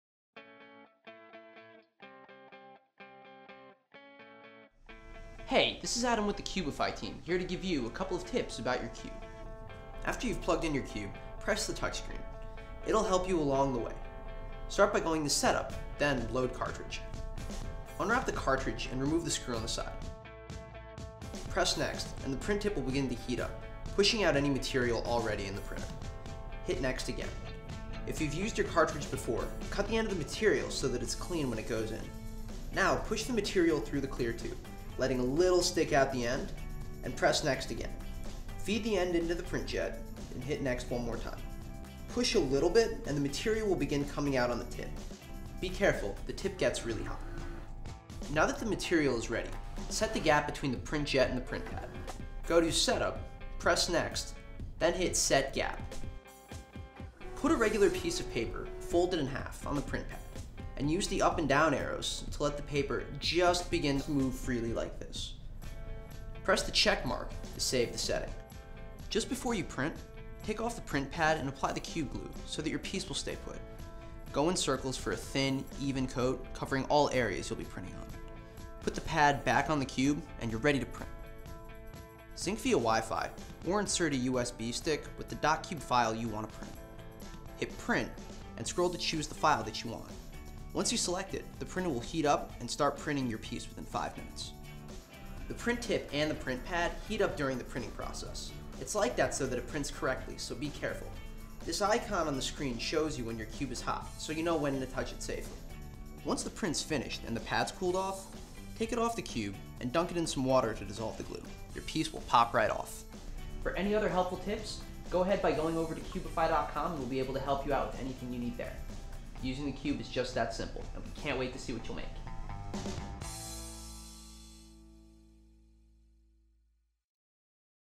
Tips for setting up your new Cube 3D Printer
In this video, Adam from the Cube team shows you how to set up your Cube 3D Printer, how to load the material cartridge and get started printing your own 3D creations.